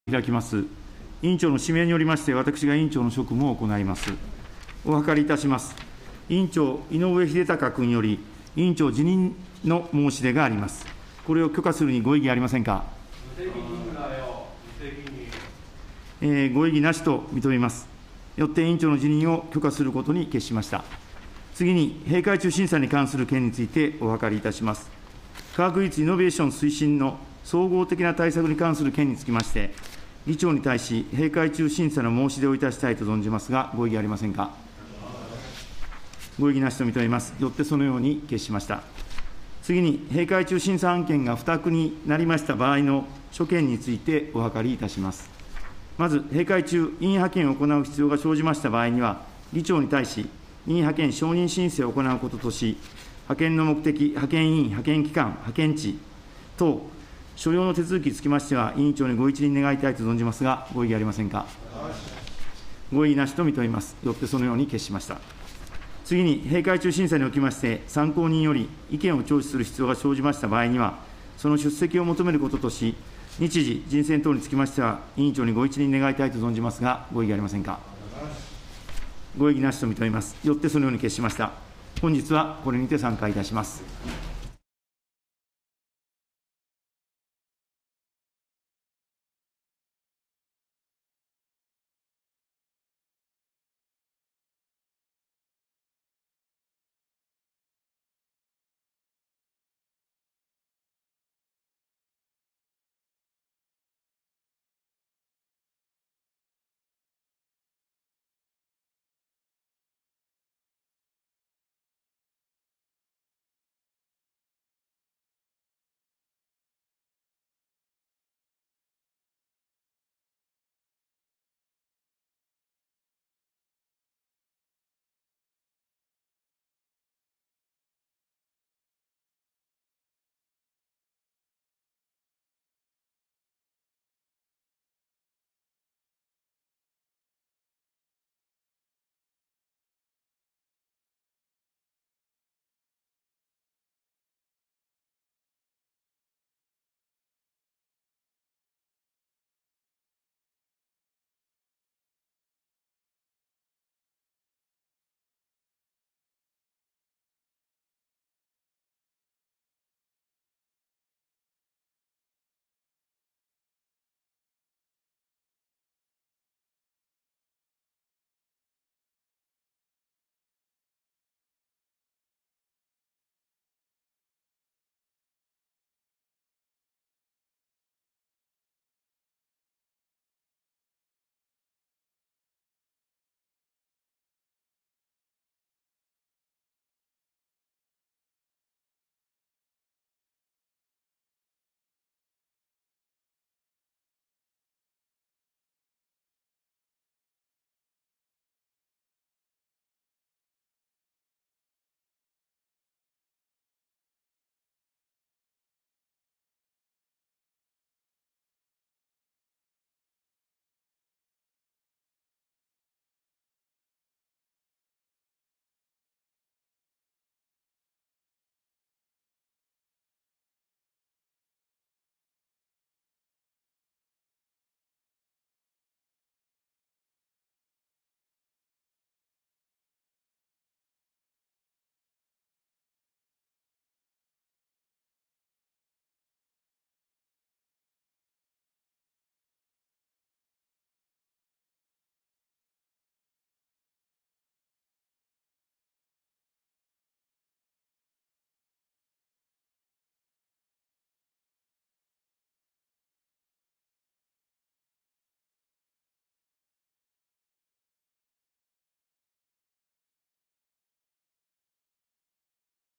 衆議院 2021年12月21日 科学技術特別委員会 #01 西村康稔（科学技術・イノベーション推進特別委員会理事）
西村康稔 科学技術・イノベーション推進特別委員会理事
2021年12月21日 衆議院 科学技術特別委員会

【まとめ 再生リスト】 2021年12月21日 衆議院 科学技術特別委員会

また、西村康稔さんの過去質疑まとめは、↓↓↓を参照してください。